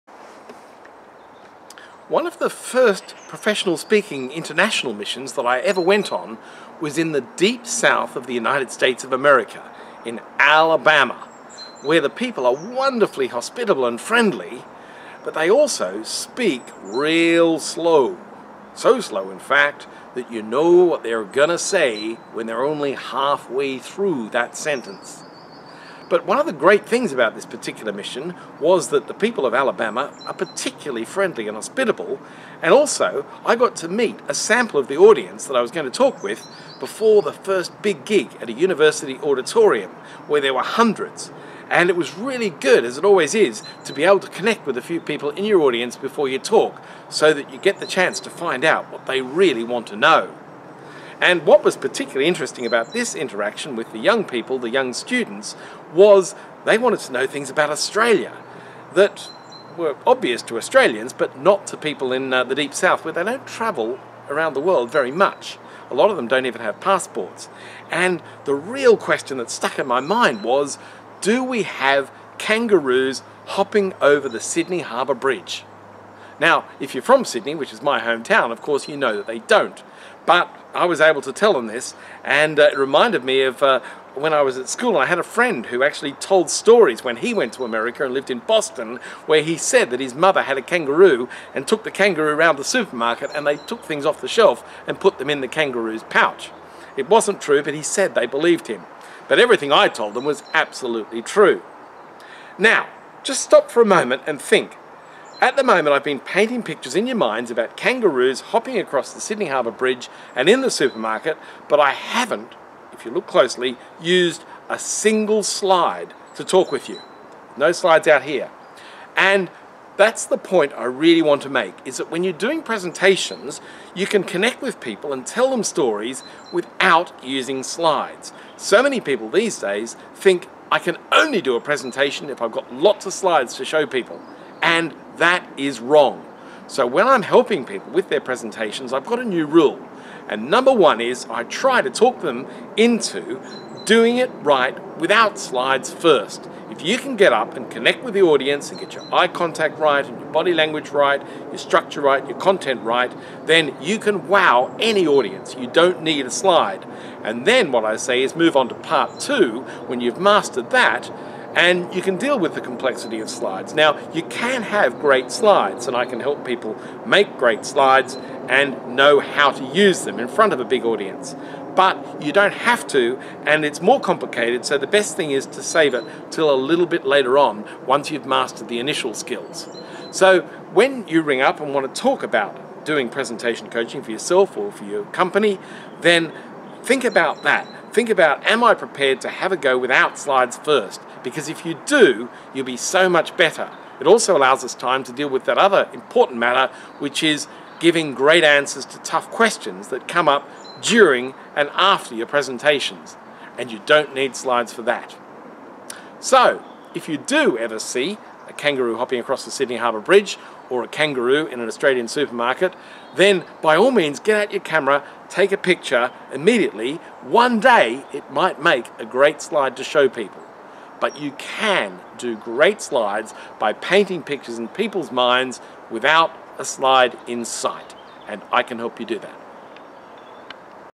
Presenting Without Slides - You Can Do It!
Michael Dodd says it's ideal to master presentation skills without involving slides - and then learn how to make and deploy slides with great effect.